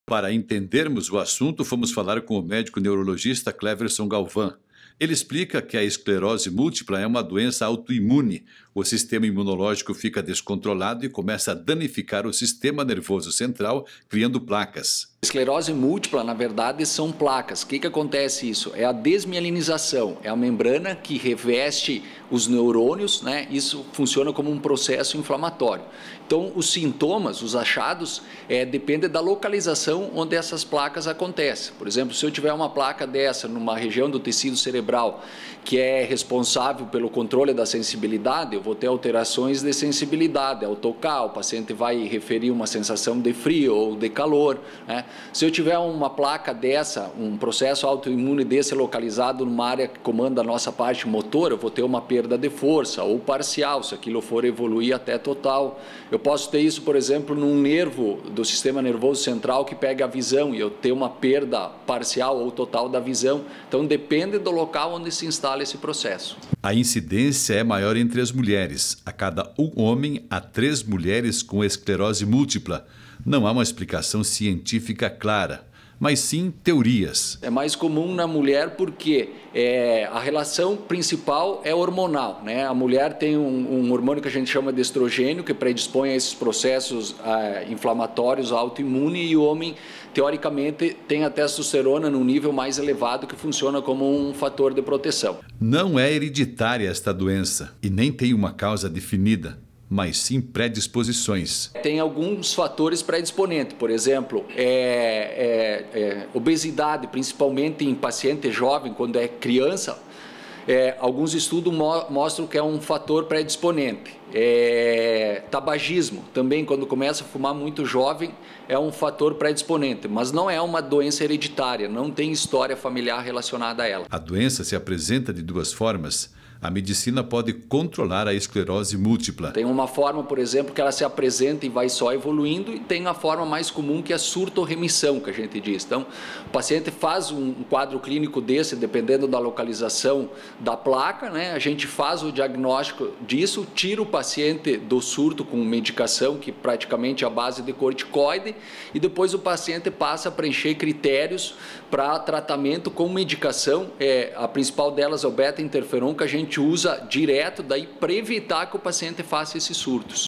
Esclerose múltipla: a doença que atinge mais mulheres que homens
Saúde: Hoje, 30 de agosto é o Dia Nacional de Conscientização sobre a esclerose múltipla, doença que atinge mais mulheres que homens.